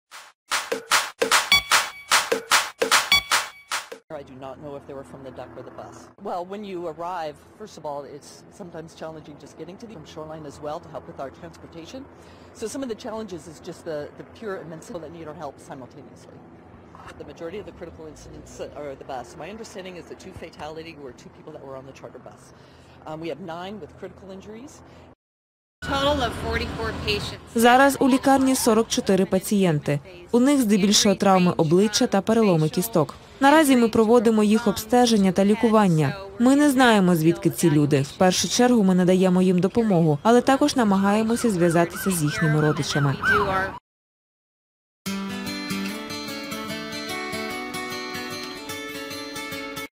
ОЧЕНЬ Страшная авария в США/ Very terrible accident in the United States
Be aware of everything in the world. The most interesting and latest news. Будьте в курсе всего в мире. Самые интересные и свежие новости со всего МИРА.
На мосту зіштовхнулись два автобуси — туристичний та рейсовий. Усі загиблі — це іноземні студенти.
50 пасажирів поранені, серед них 12 у важкому стані. Зі слів очевидців, аварія сталася по вині водія туристичного автобусу, який не впорався з керуванням.

On the bridge faced two buses - travel and shuttle. All the dead - are foreign students.
50 passengers were injured, including 12 in serious condition. According to eyewitnesses, the accident occurred on the wine tourism bus driver who lost control.